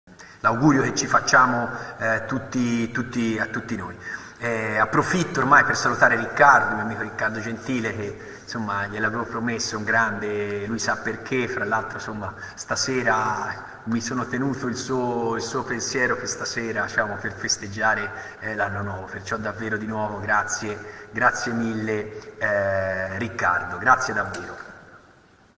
Un saluto dal grande sindaco di Prato Matteo Biffoni.
il sindaco di Prato, Matteo Biffoni che mi saluta durante una delle sue tante dirette Facebook e Instagram